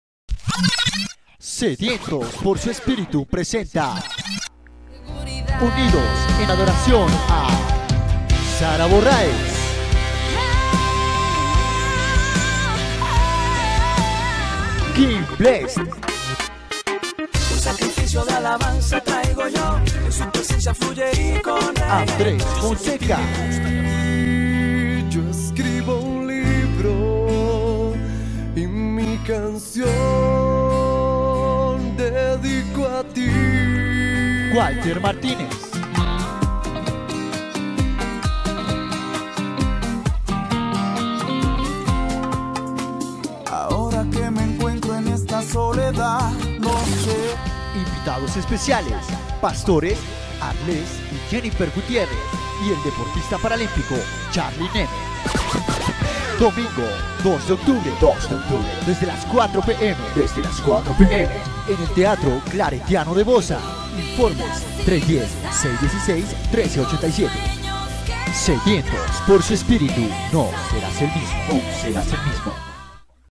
PROMO SEDIENTOS POR SU ESPIRITU
SEDIENTOS POR SU ESPIRITU SERA EL DIA 2 DE OCTUBRE DE ESTE AÑO DESDE LAS 4 PM EN EL TEATRO CLARETIANO, CON LA PARTICIPACION DE SARA BORRAEZ...KING BLESSED...ANDRES FONSECA...WALTER MARTINEZ...Y CON LA PARTICIPACION ESPECIAL DE LOS PASTORES ARLEX Y JENNIFER GUTIERREZ Y CHARLY NEME...INFORMES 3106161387 O POR FACEBOOK COMO ANDRES FONSECA BANDA OFICIAL